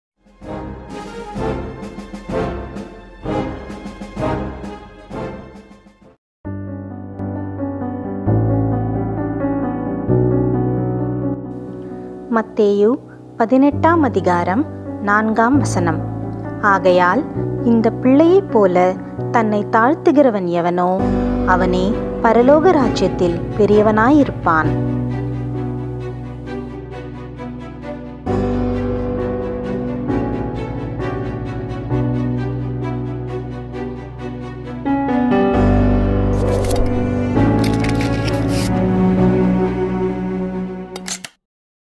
அதுவே உன் வாழ்வின் சொத்து Nov29 | "மத்தேயு 18:4"
தினமுத்து - ஒரு நாள் ஒரு வசனம் உங்கள் வழியை உருவாக்குகிறது. கடவுளுடைய வார்த்தை வாழ்க்கையில் ஒரு மாற்றத்தைக் கொண்டுவருகிறது. கேளுங்கள், தியானியுங்கள், ஆசீர்வதிக்கப்படுங்கள்.

உங்கள் ஆதரவோடு இன்னும் வேத வசனங்களை சுவாரஸ்தியதோடு கொண்டுவர முயர்சிப்போம்